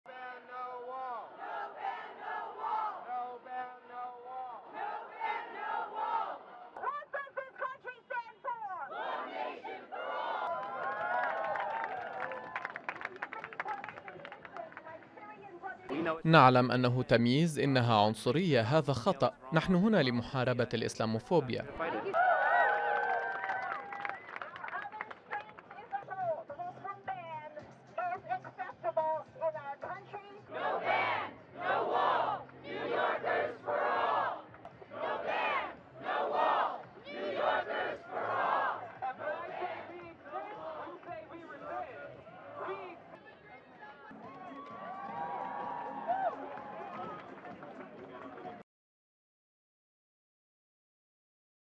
مرسوم ترامب بشأن الهجرة دخل حيز التنفيذ
دخلت القيود الجديدة للدخول إلى الولايات المتحدة بموجب المرسوم ضد الهجرة الذي أصدره دونالد ترامب حيز التنفيذ مساء الخميس ما أشاع قلقا بين مسافري الدول المعنية والجمعيات المدافعة عن اللاجئين.تقرير بالصوت الطبيعي فاني تكسر/محمد الغندور